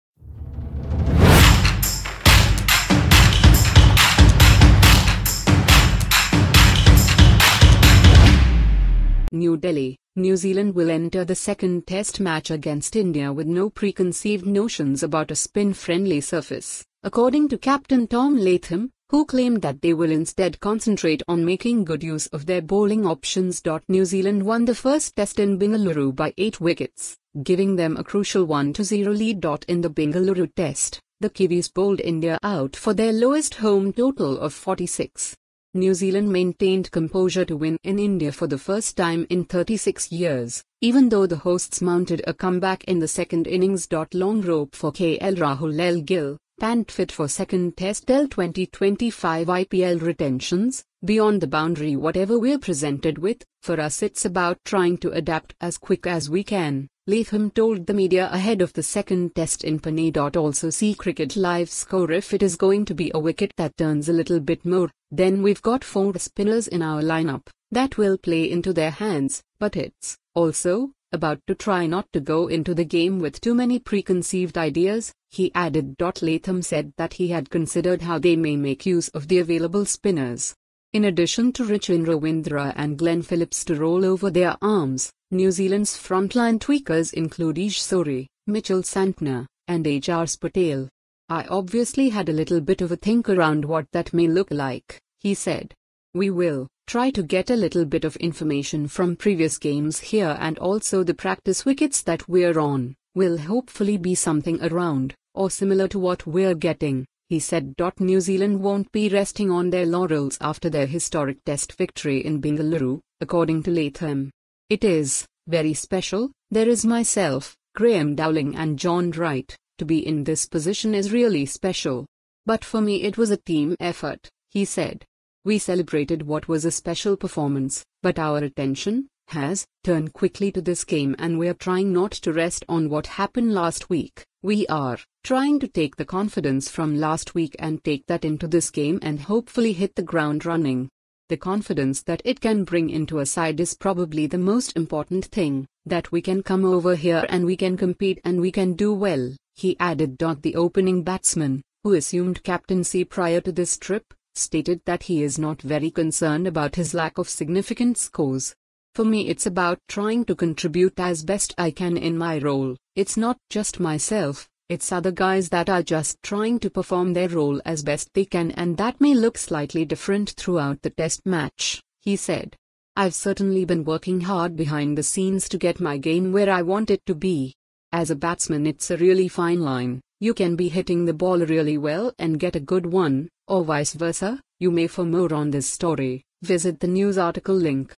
'If it is going to be a wicket...': Latham ahead of 2nd Test against India
'If it is going to be a wicket...': Latham ahead of 2nd Test against India⁣ For up-to-date Indian cricket news